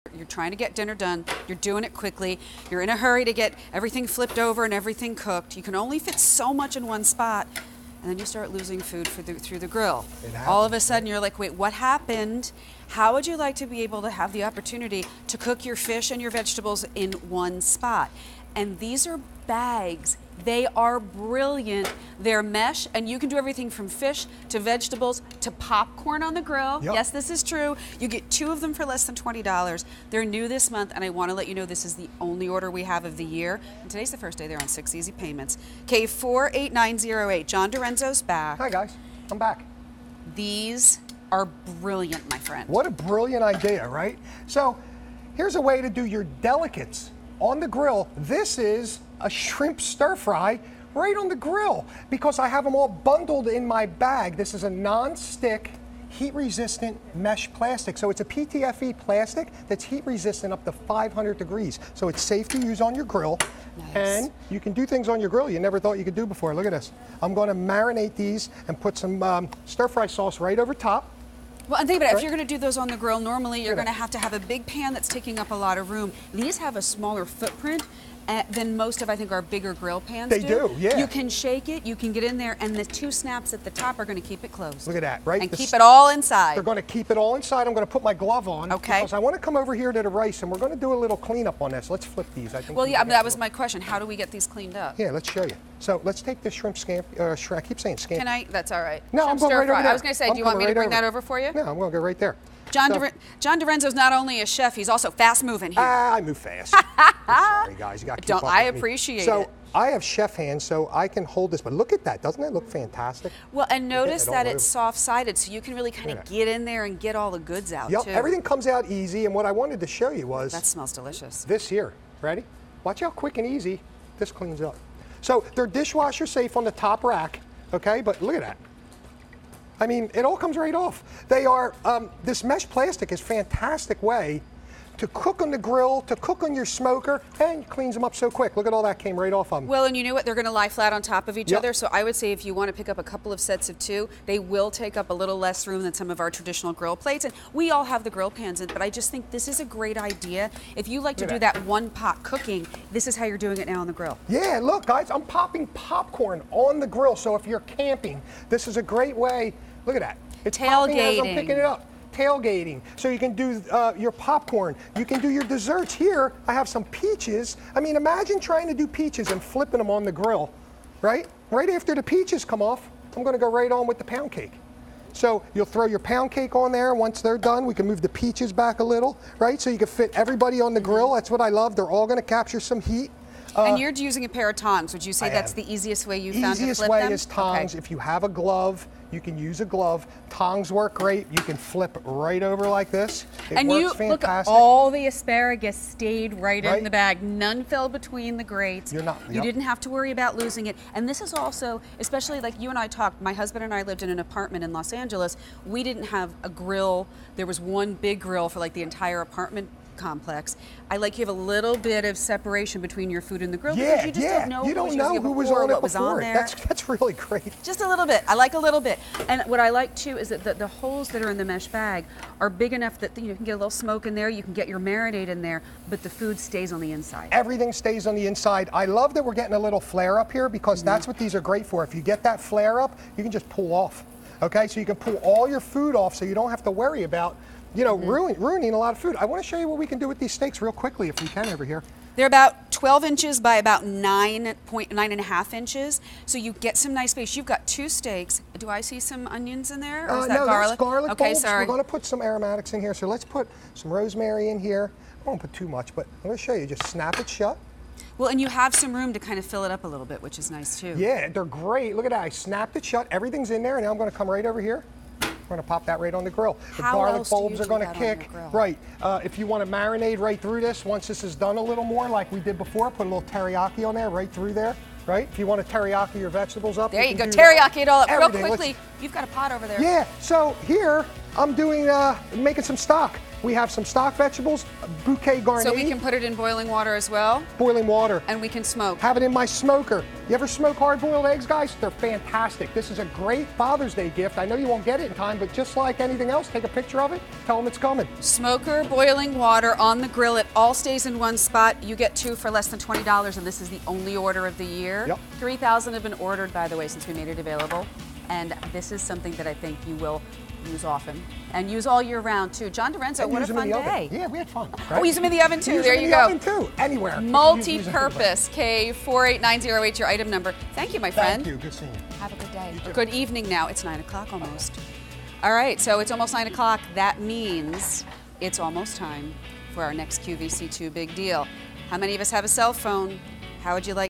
Set of 2 Snap-to-Close Large Mesh Grill Bags on QVC
Set of 2 Snap-to-Close Large Mesh Grill Bags

Cookout time, and the grillin's so easy with these mesh bags that keep shrimp, hot dogs, and other bites from falling through the grill grates.